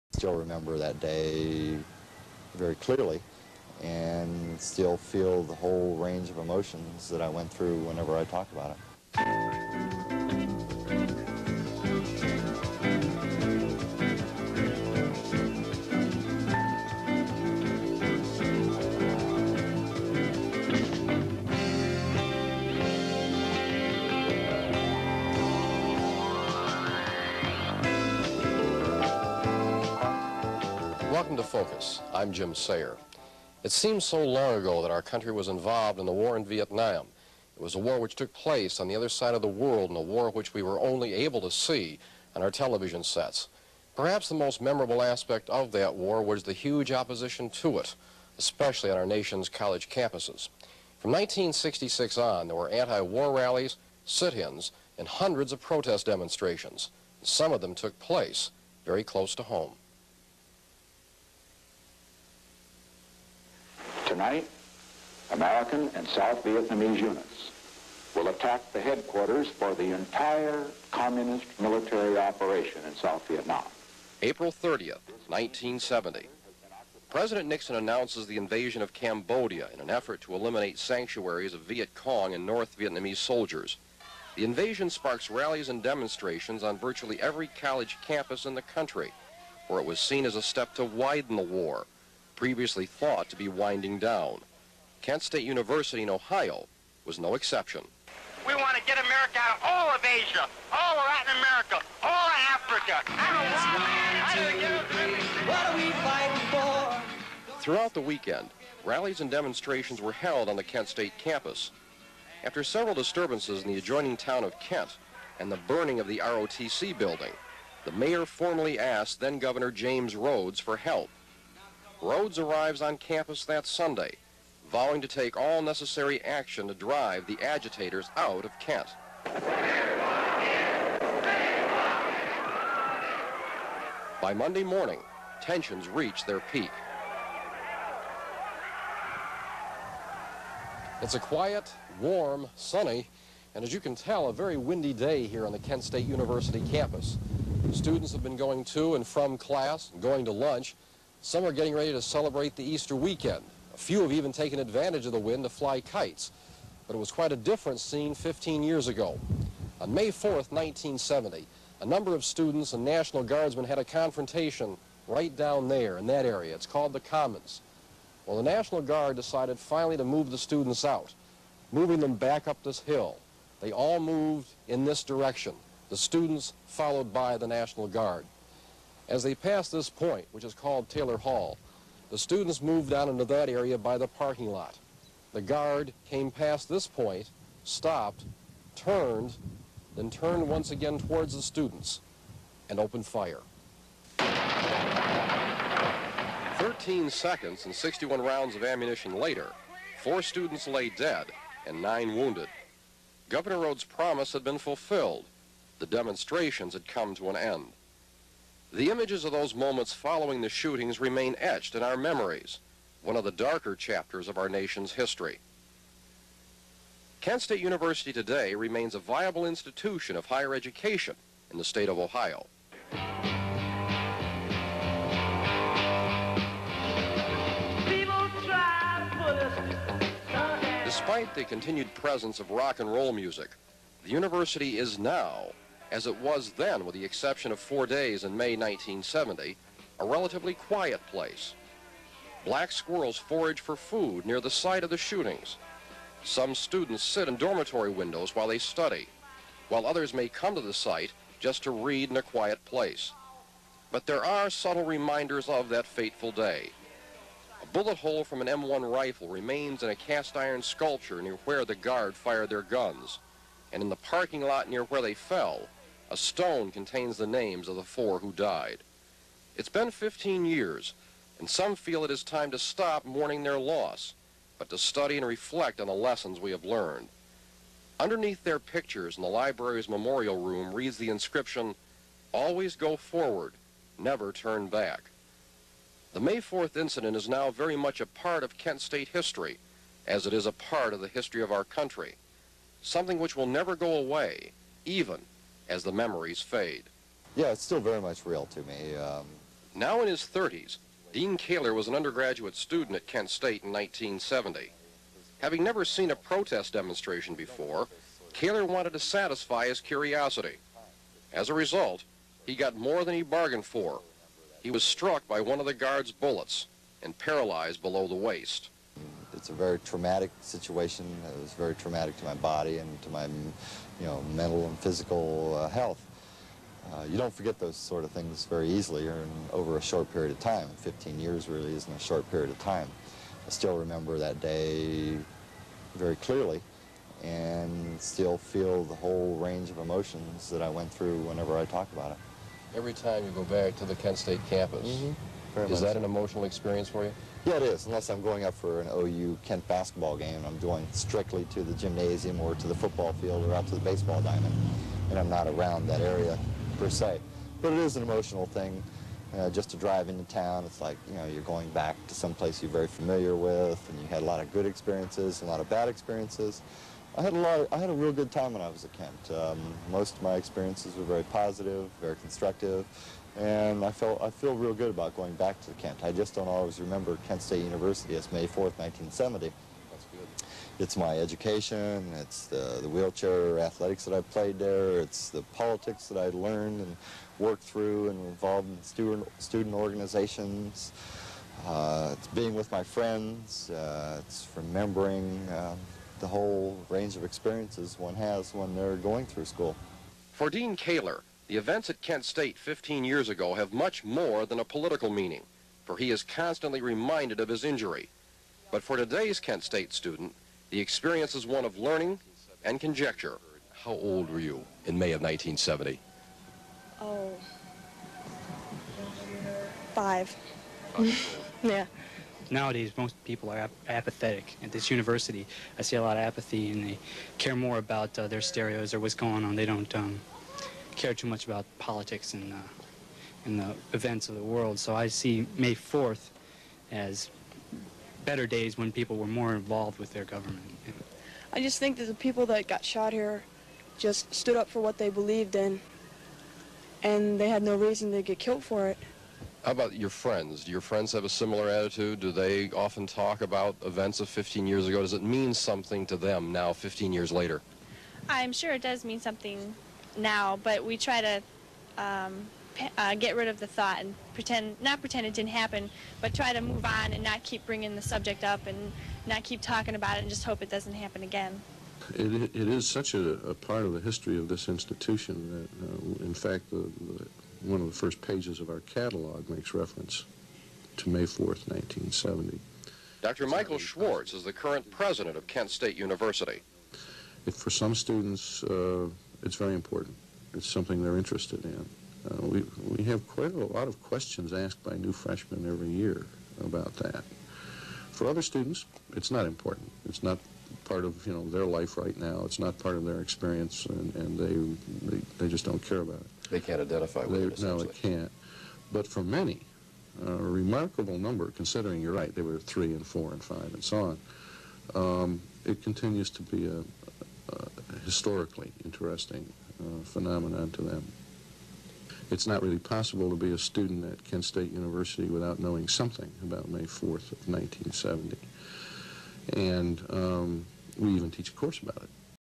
FOCUS: 15th Anniversary of Kent, Part 1
Focus presentation of Kent State, the town and campus, 15 years after that fateful day May 4, 1970.